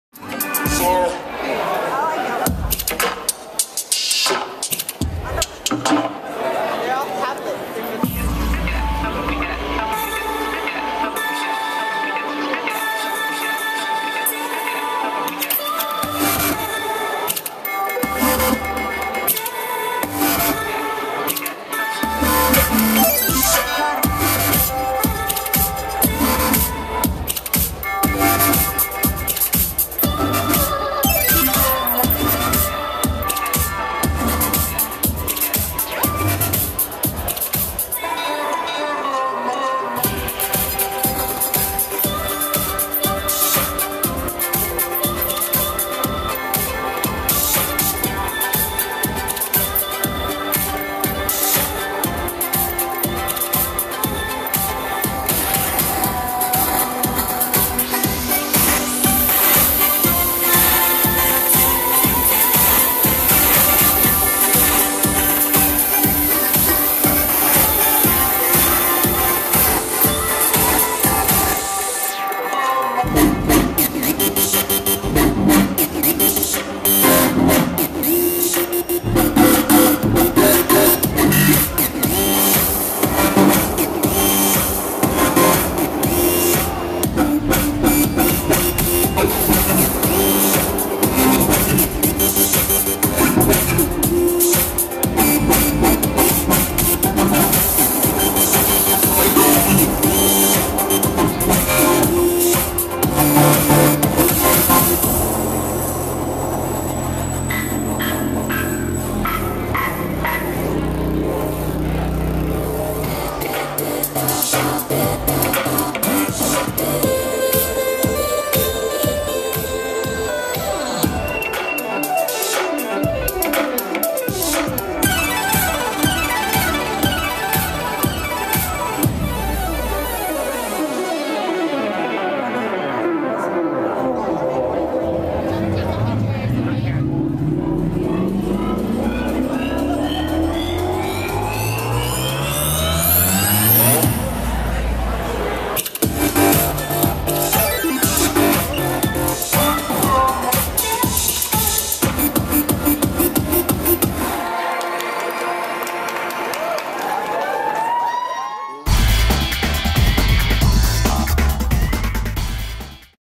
Movement Factory Qualcomm Snapdragon DJ Mix
As if the venue wasn't cool enough, the DJs used an array of Snapdragon tablets to jam up an insane music set with live visuals to match. Awesome!